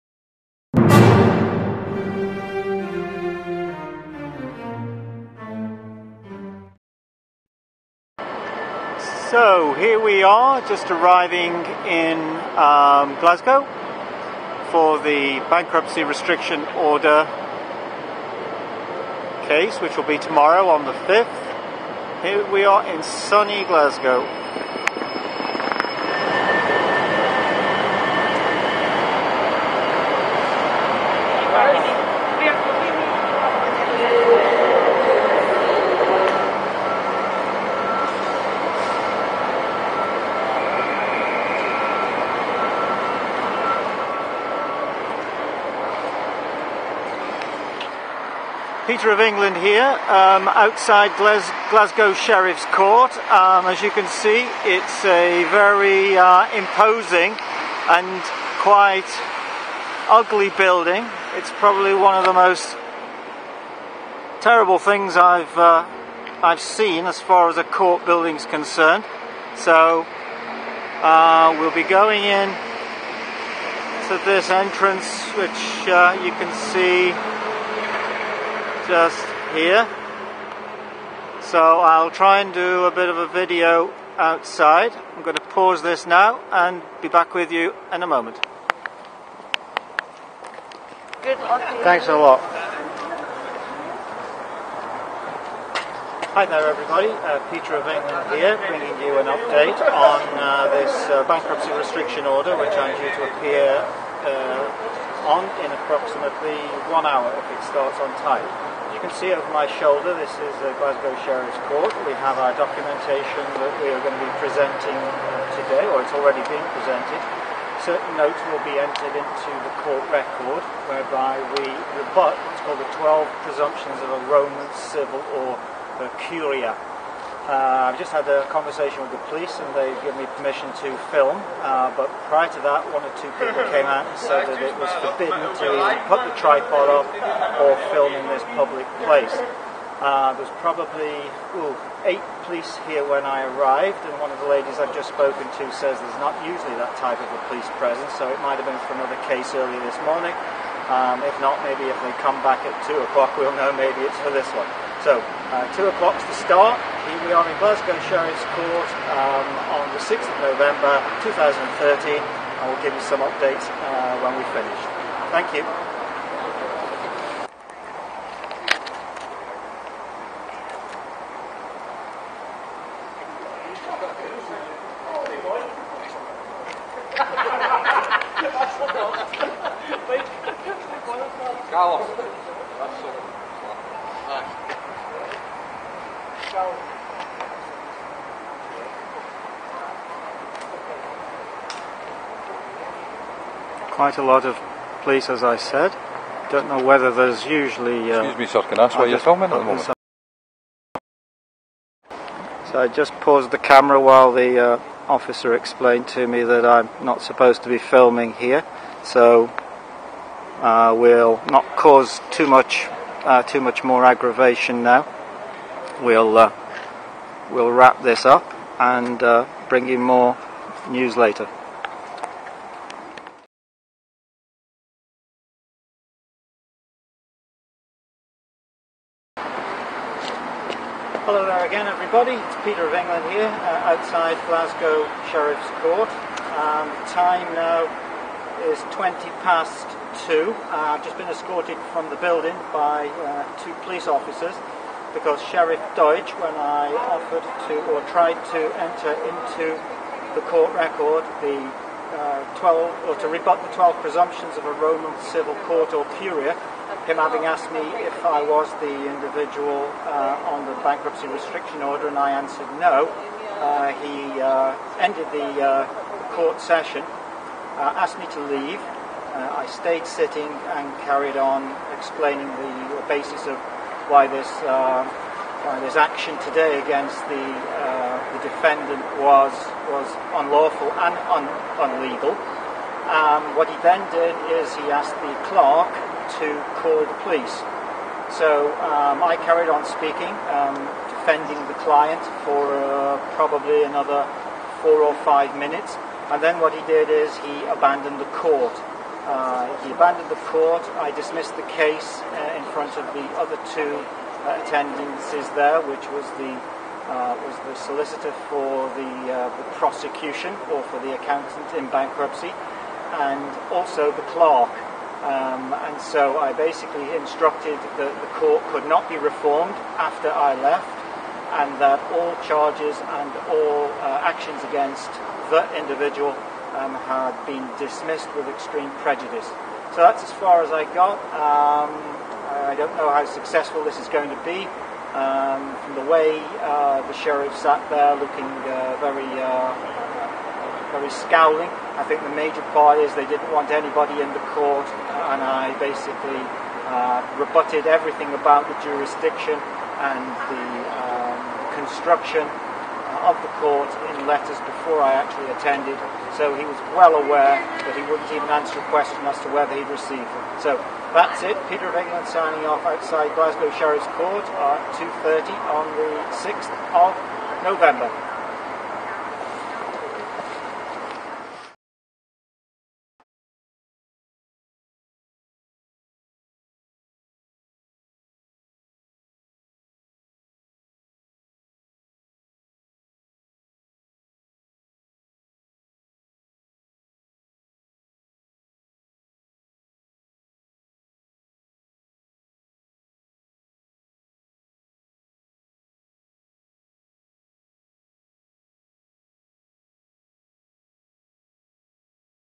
Sheriff 0 Peter of England 1 (0:2 on aggregate)
Defence of a Freeman Legal Services client in Scotland on 6th November 2013
The formula is almost complete - this one was particularly difficult in that the Sheriff ordered me out of the court within the first minute of the commencement. However, I stood my ground and he withdrew before me so you tell me who won?